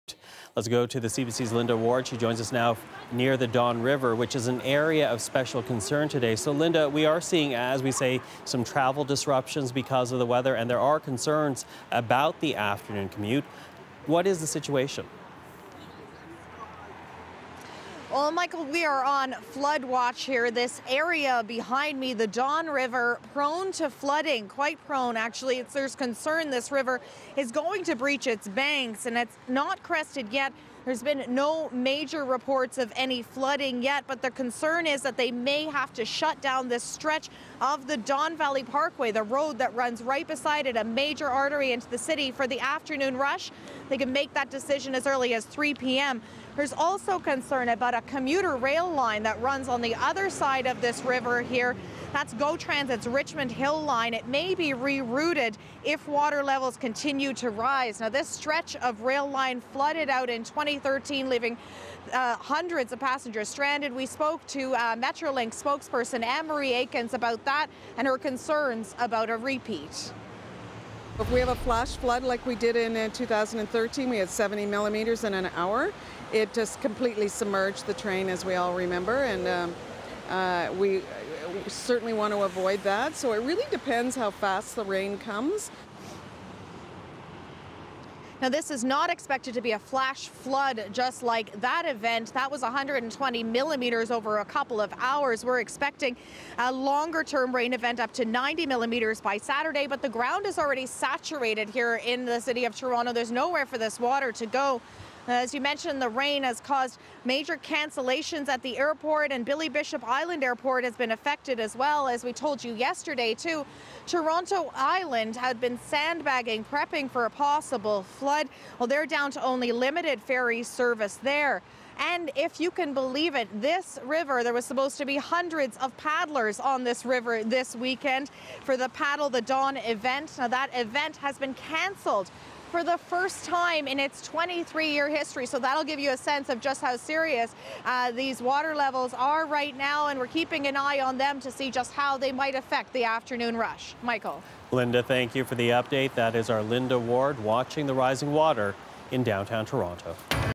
Rain could close Toronto highway
As the rain continues to fall Friday morning commuters and transit passengers are watching river levels closely.